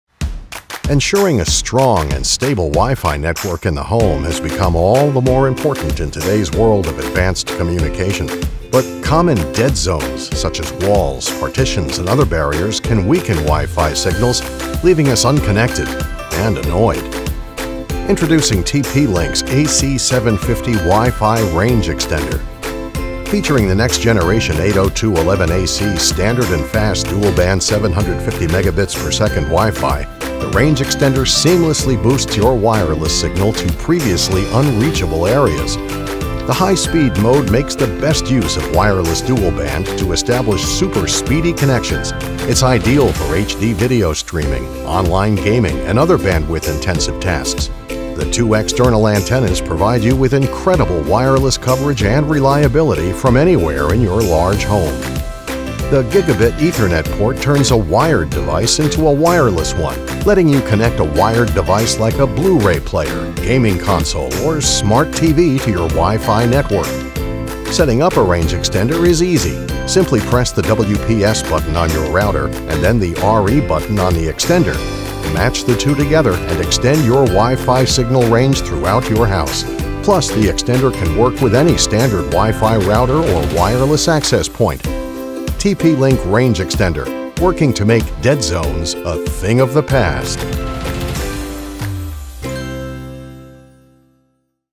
TP-Link AC750 Range Extender Introduction Video
AC750 Range Extenders

The AC750 expands your network and minimizes "dead zones" using high-performance antennas and 802.11ac, the latest Wi-Fi technology. The extender's Gigabit Ethernet port connects wired devices like smart TVs and gaming consoles to your network.

FEATURES:
• Strengthens your router’s Wi-Fi signal to minimize coverage “dead zones”
• Uses 802.11ac, the latest, most powerful Wi-Fi technology
• Dual 5GHz (433Mbps) and 2.4GHz (300Mbps) bands
• High Speed mode combines Wi-Fi bands to maximize performance
• Color-changing Intelligent Signal Indicator light gauges Wi-Fi strength to help you find the perfect set-up spot

THIS VIDEO APPLIES TO THESE MODELS: RE210 and RE200.